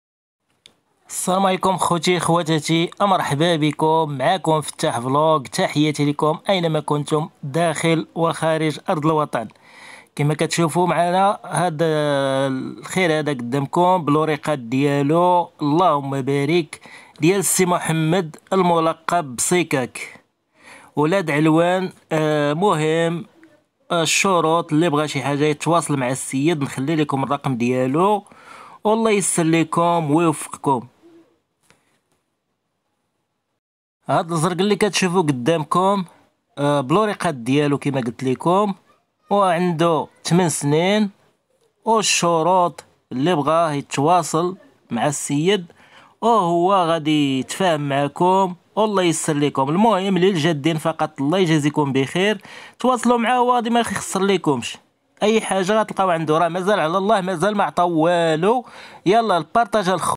عود زرك البيع ديال سي محمد سيكاك دوار ولاد علوان السهول ناضي بلاوراق
الخوت عود ازرق للبيع مقدر  عربي
حصان عربي مقدر للبيع مزال معطاو والو يالله توكلو علا الله وشكرا علا البارطاج